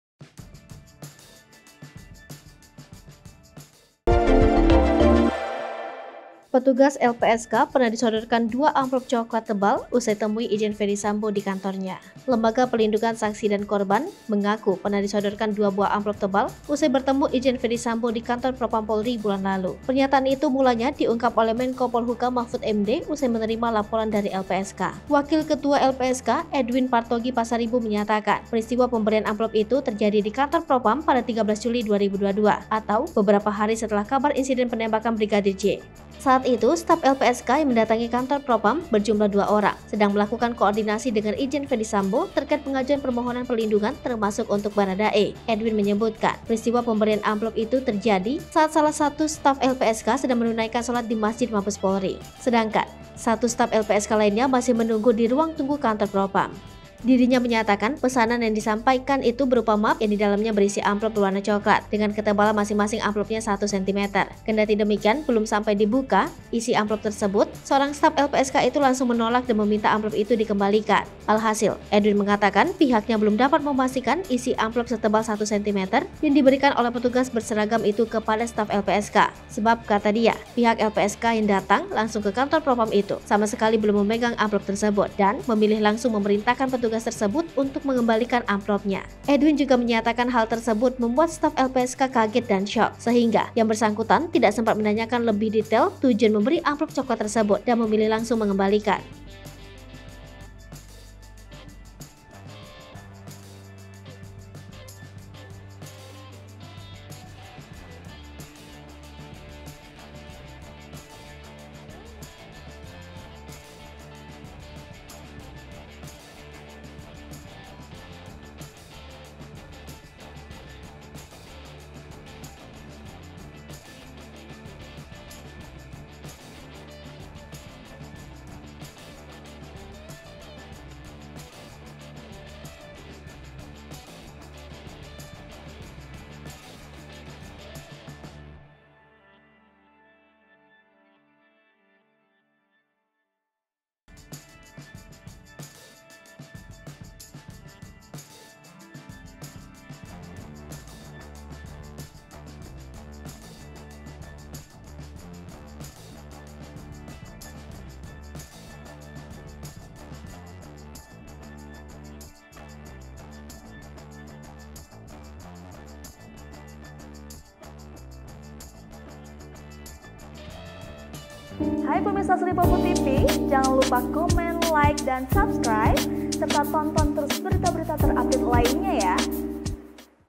Usai Temui Ferdy Sambo, LPSK Diduga Disuap 2 Amplop Tebal
Wakil Ketua LPSK Edwin Partogi Pasaribu menyatakan, peristiwa pemberian amplop itu terjadi di Kantor Propam pada 13 Juli 2022, atau beberapa hari setelah kabar insiden penembakan Brigadir Nofriansyah Yosua Hutabarat mencuat.

"Pada saat kesempatan tersebut, salah seorang staf berseragam hitam dengan garis abu-abu, menyampaikan titipan atau pesanan 'Bapak' untuk dibagi berdua di antara Petugas LPSK," beber Edwin.

Dirinya menyatakan, pesanan yang disampaikan itu berupa map yang di dalamnya berisi amplop berwarna cokelat, dengan ketebalan masing-masing amplopnya 1 sentimeter.

Lihat informasi terkini bersama kami, di:

Palembang.tribunnews.com